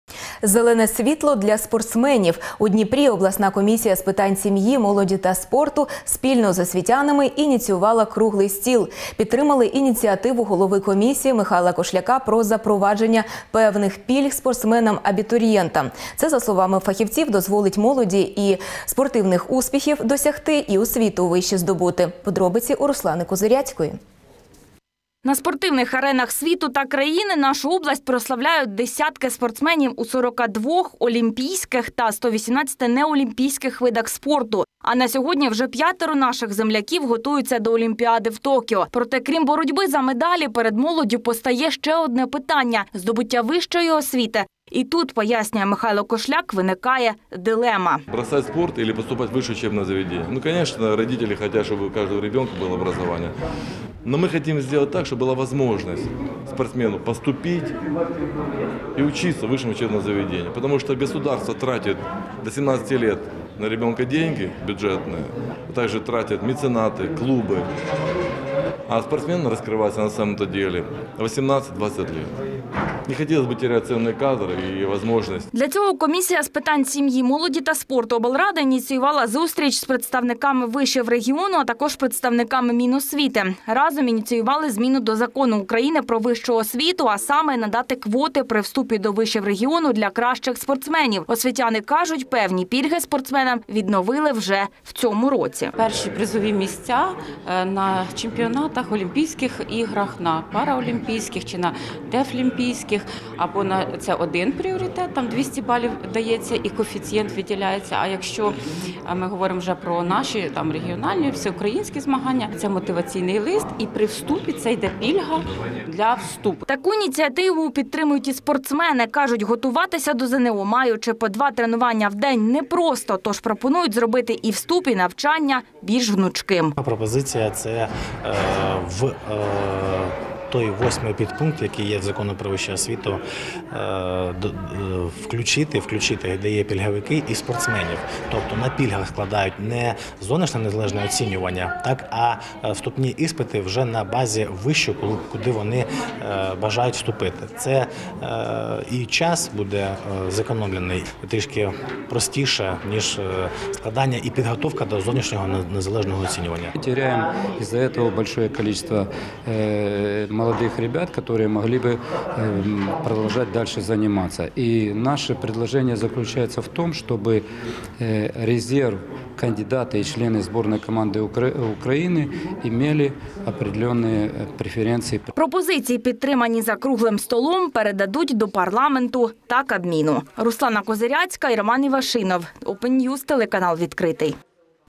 На Дніпропетровщині спортсменам допоможуть із вступом до ВНЗ
У Дніпрі обласна комісія з питань сім’ї, молоді та спорту спільно з освітянами ініціювали  певні пільги спортсменам при вступі до вишів.